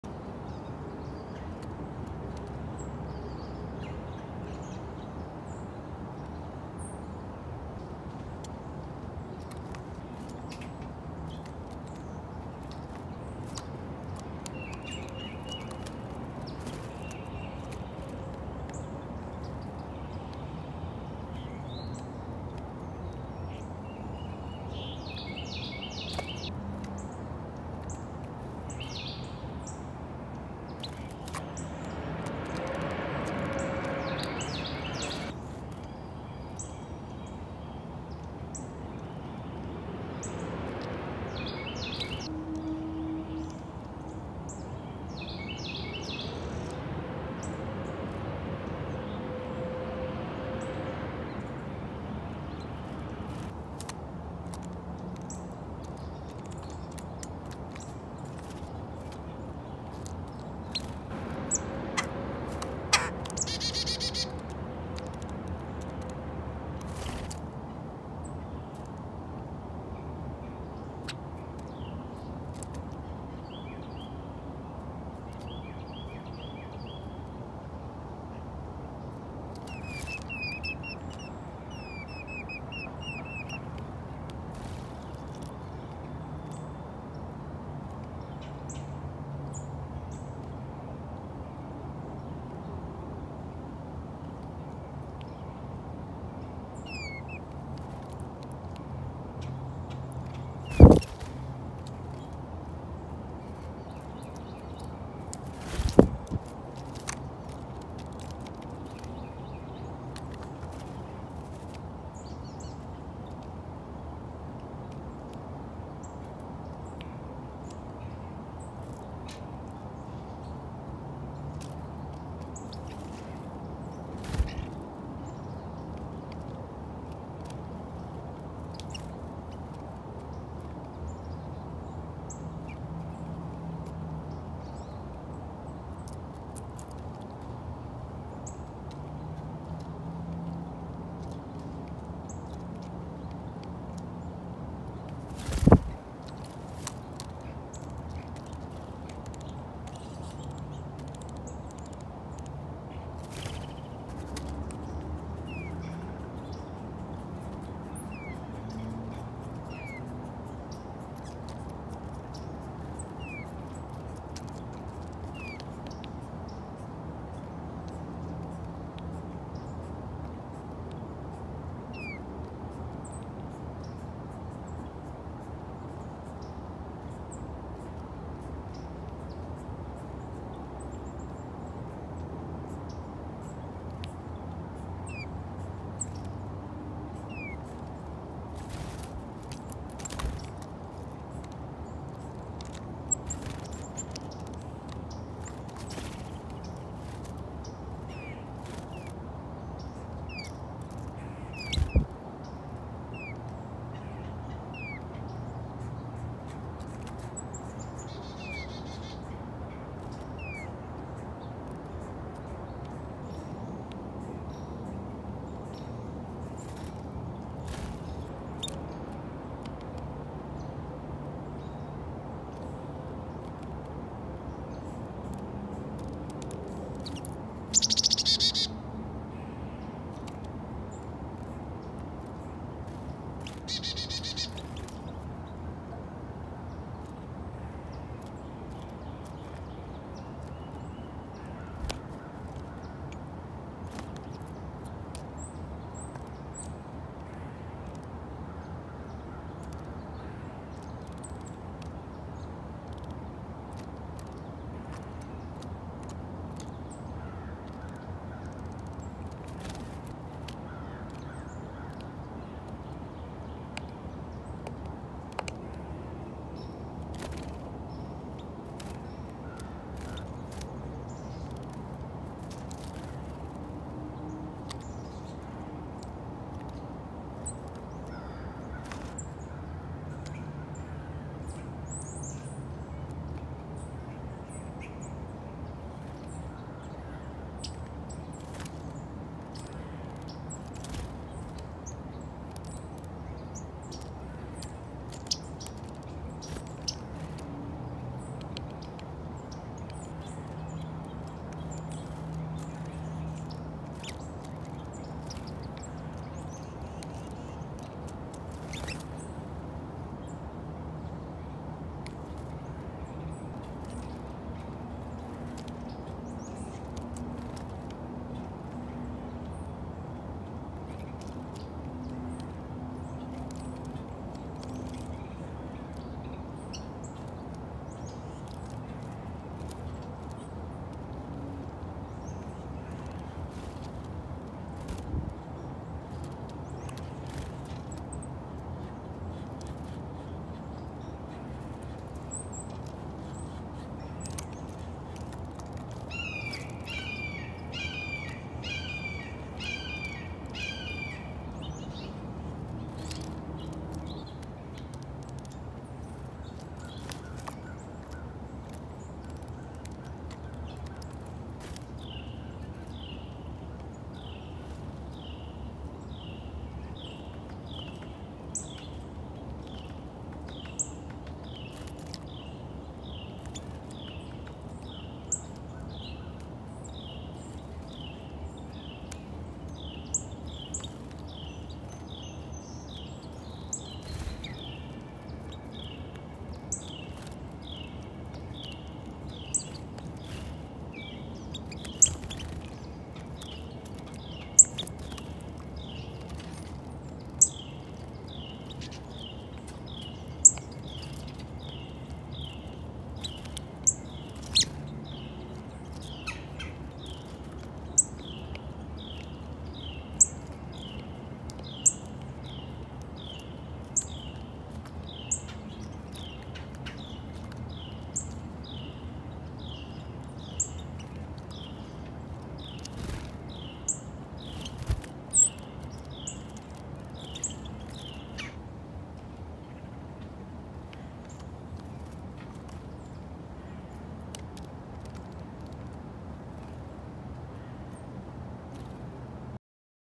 Close-up of wild birds - Winter in Georgia
HD video of birds in their natural habitat. In order of appearance today: Carolina wren, Carolina chickadee, male red-bellied woodpecker, female downy woodpecker, brown-headed Nuthatch, yellow-rumped warbler, Female and male eastern bluebirds, white-breasted nuthatch, tufted titmouse, male downy woodpecker, pine warbler, white-throated Sparrow, house finch. Lots of fun!